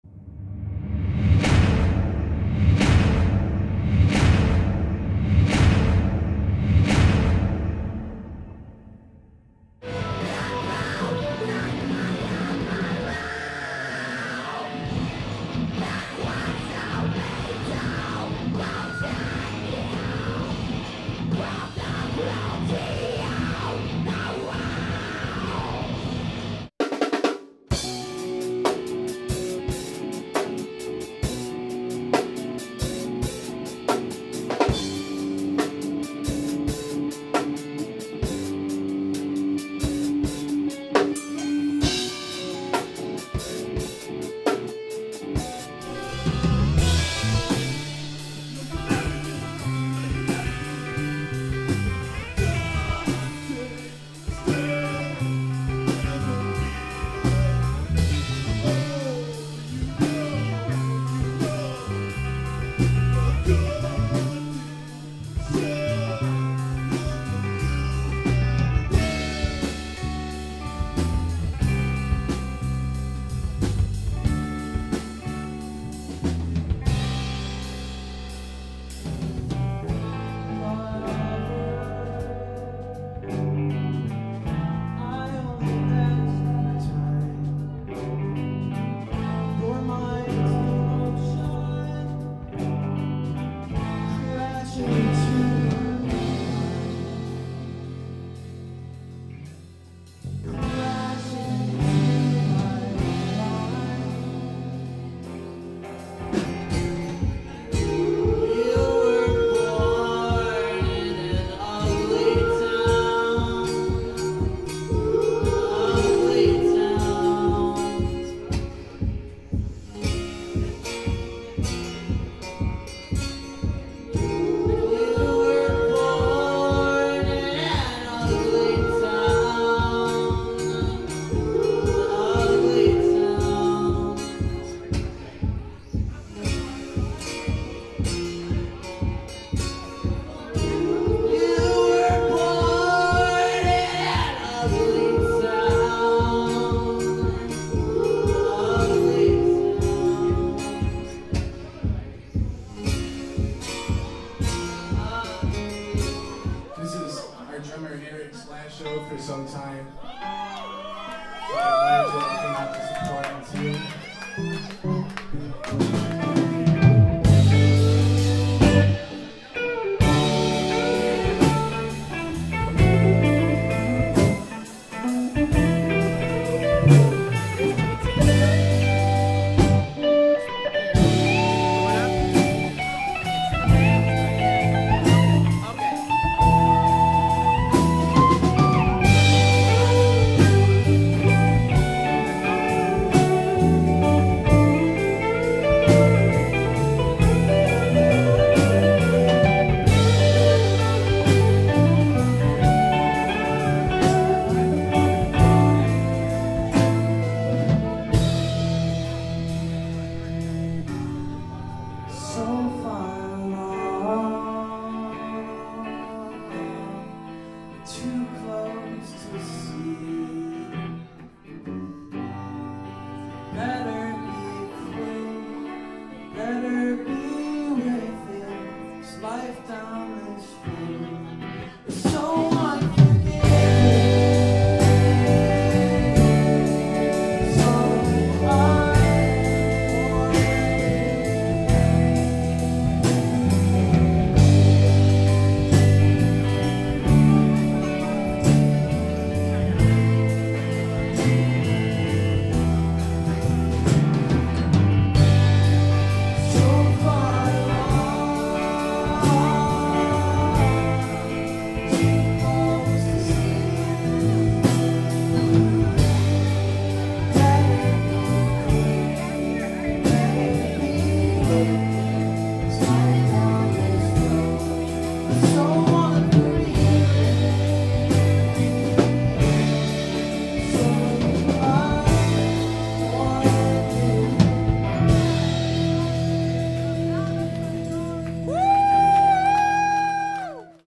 Masnema Mondays (05-14-12)
In this episode, we announce our search for a new drummer and show you some clips from a recent local Ypsilanti show.